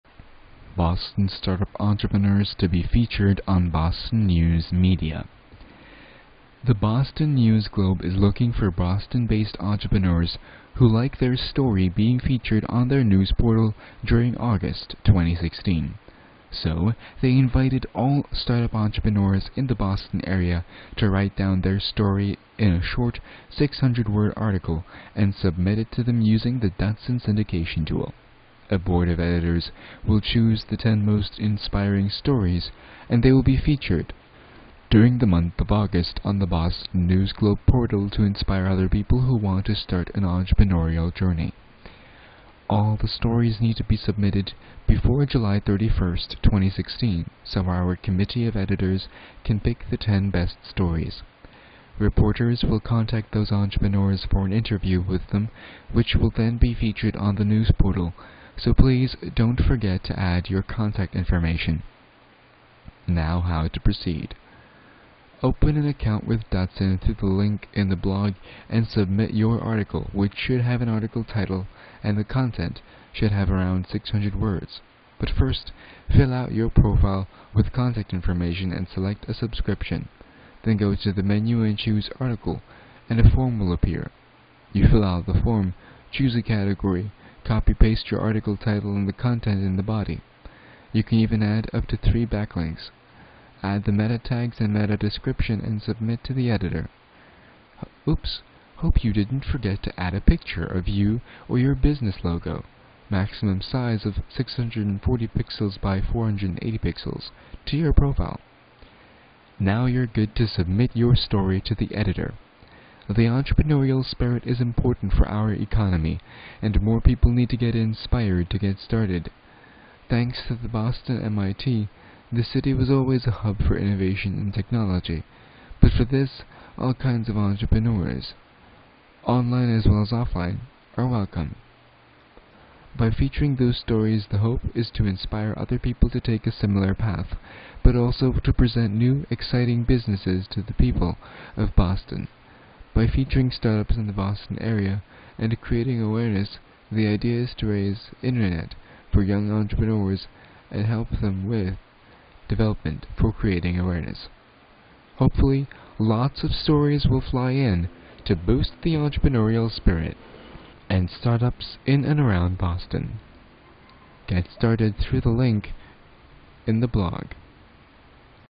Boston Start-Up Entrepreneurs To Be Featured On Boston News Media
The Boston News Globe is looking for Boston-based entrepreneurs who like their story being featured on their news portal during August 2016. So they invited all start-up entrepreneurs in the Boston area to write down their story in a 600 words article and submit it to them using the DatSyn syndication tool. A board of editors will choose the 10 most inspiring stories and they will be featured during the month of August on the Boston News Globe portal to inspire other people who want to start an entrepreneurial journey.